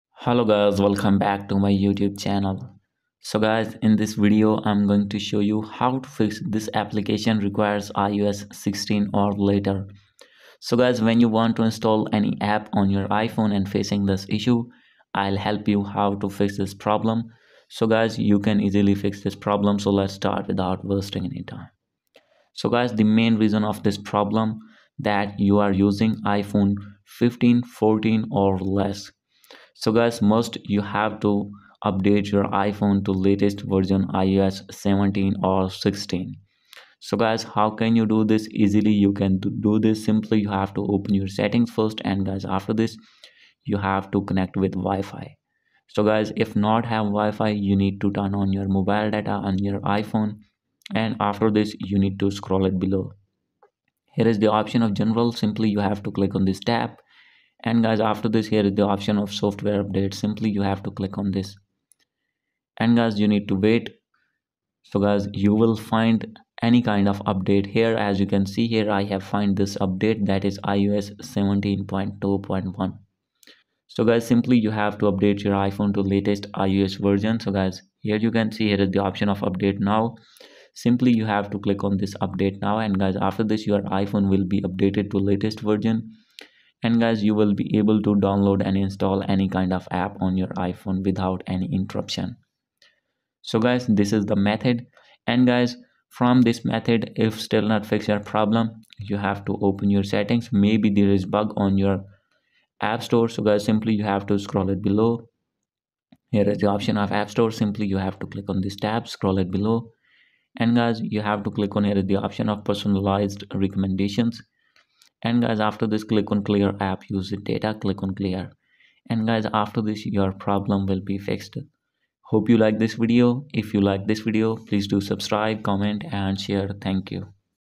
This Application Requires iOS 16.0 or Later You Must Update to iOS 16.0 to Download This App | 2024
In this video, I will help you to fix This Application Requires iOS 16.0 or Later You Must Update to iOS 16.0 to Download This App. Please watch till the end.